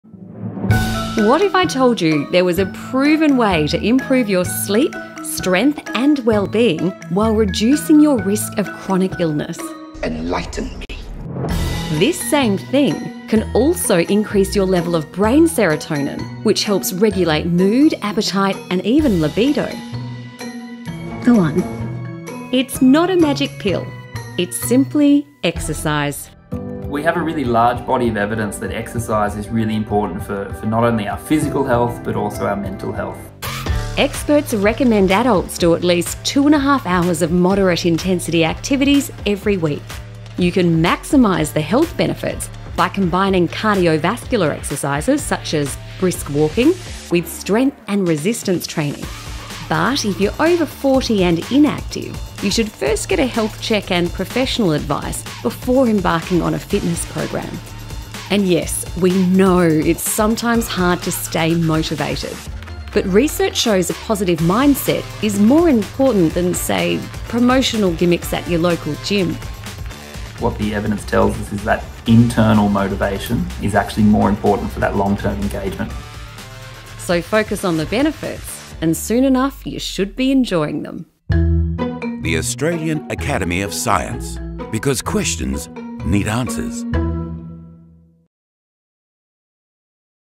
The proven way to better sleep and mood
Exercise is the one ‘fix it’ when comes to improving your sleep, strength, and wellbeing.  Here is where to find the motivation to get up and move.